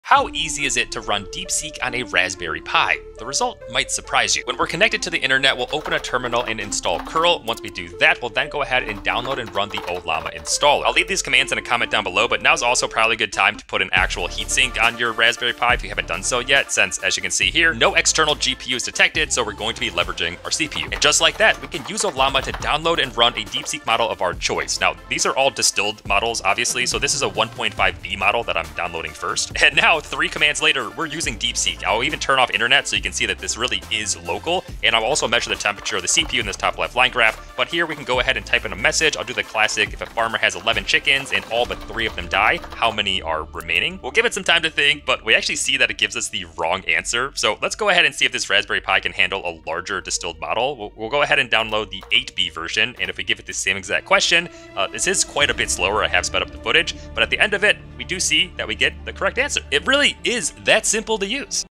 How to install DeepSeek on a Raspberry Pi
How to install DeepSeek on a Raspberry Pi for free offline, local use!